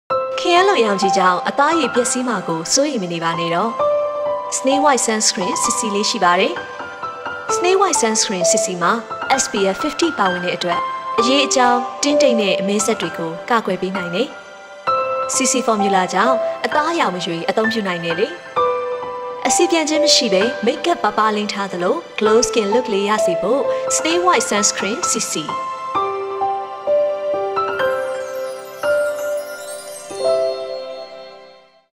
𝐒𝐧𝐚𝐢𝐥𝐰𝐡𝐢𝐭𝐞 𝐒𝐮𝐧𝐬𝐜𝐫𝐞𝐞𝐧 𝐂𝐂
ခရမ်းလွန်ရောင်ခြည်ကြောင့် အသားအရေပျက်စီးမှာကို စိုးရိမ်နေတဲ့သူတွေအတွက်... 𝐒𝐧𝐚𝐢𝐥𝐰𝐡𝐢𝐭𝐞 𝐒𝐮𝐧𝐬𝐜𝐫𝐞𝐞𝐧 𝐂𝐂 လေးရှိပါတယ်
Snailwhite Double Boosting Serum  ( TV Commercial)
Client - Dr.Kyawt Thet Lwin
Mixing & Master - Ye Yint (G Fam)
Music Produced  - Ye Yint (G Fam)
Cast - Thinzin Nwe Win
Makeup - Jay Jay
Producer- Ye Ko (Media Waves)
Production Manager- Thein Zaw Htut
Focus - Pa Laung
DOP - Ye Naung
Post Production- Media Waves
Director - Cthu Cho Gyi
Production - Media Waves